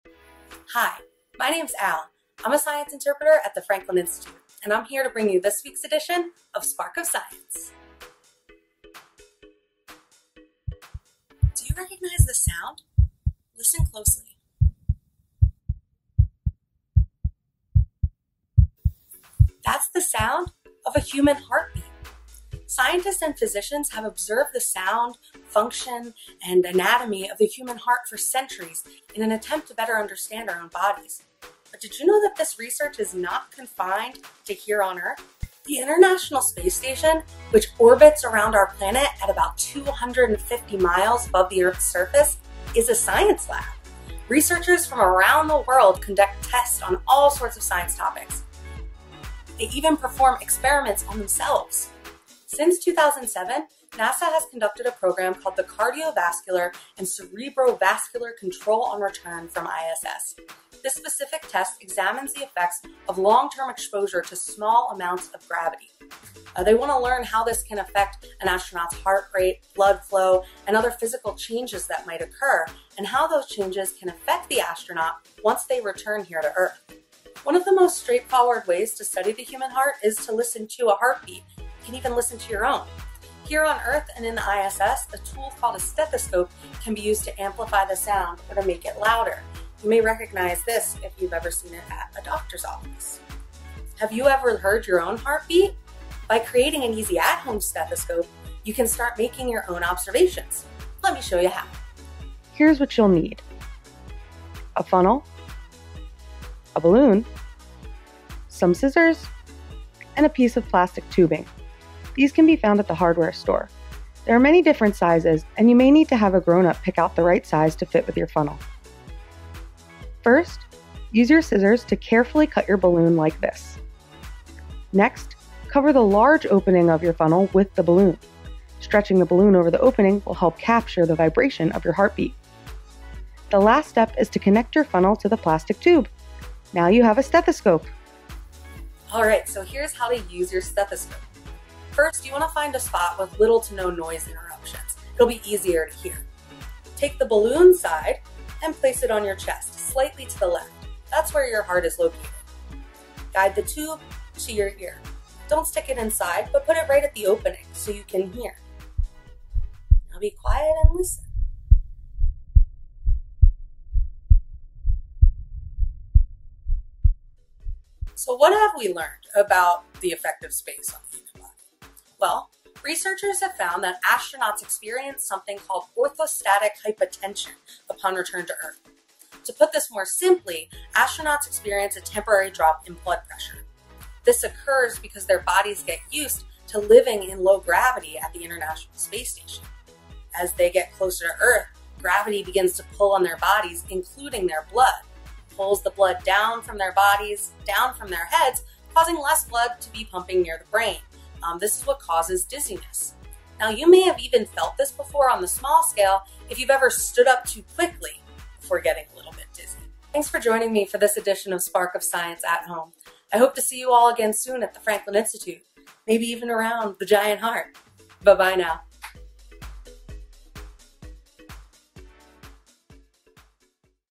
SparkofScience: Stethoscope
We know that going into space affects the bodies of astronauts. So how does it affect their hearts? And how can you listen to your own? Science Interpreter Al is pumped and ready to show you!